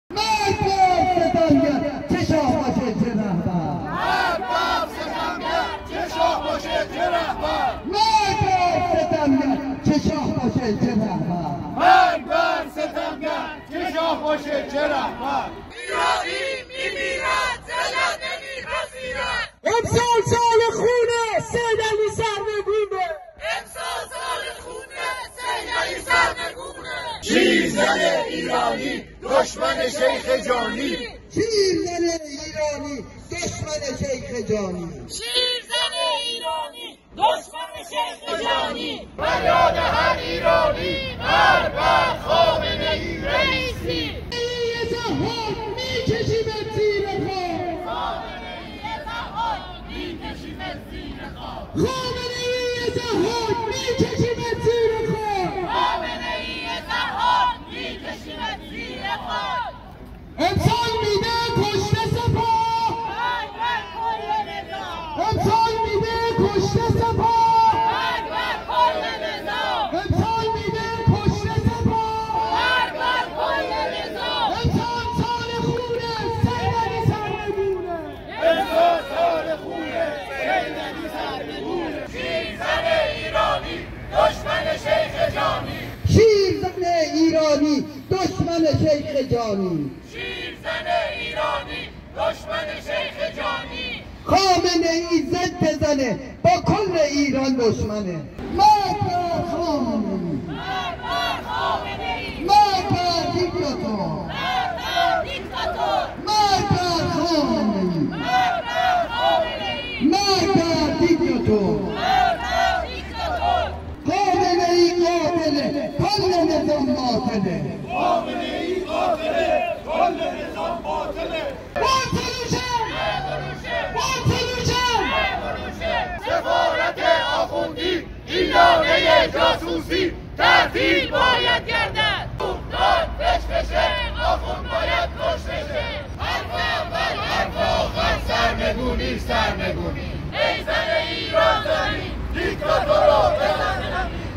Berlin, Germany—November 26, 2022: MEK Supporters Rally in Support of the Iran Protests.
Berlin, Germany—November 26, 2022: Freedom-loving Iranians and supporters of the People's Mojahedin Organization of Iran (PMOI/MEK) held a rally at the Brandenburg Gate (Brandenburger Tor) in solidarity with the Iranian people's uprising.